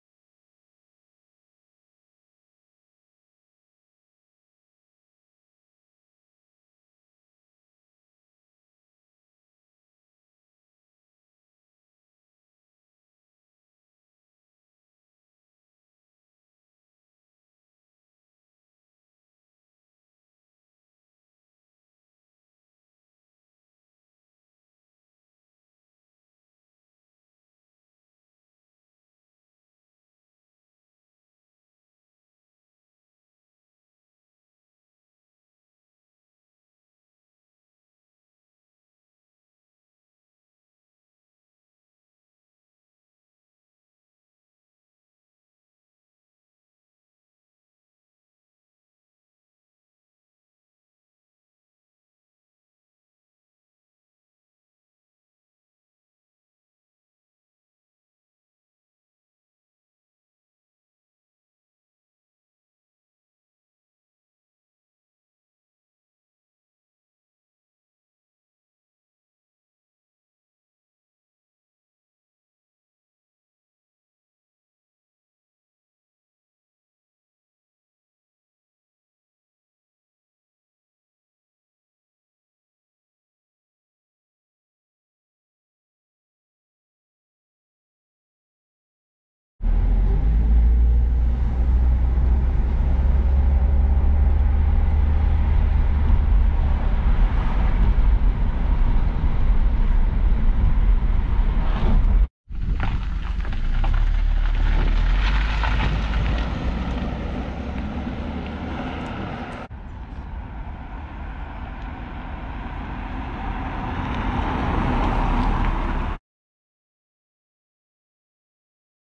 1991 Mercedes 190E 2.0 Auto
Anglia Car Auctions Kings Lynn are pleased to offer this 1991 Mercedes 190E 2.0 Auto as an entry in our August 28th and 29th classic car auction.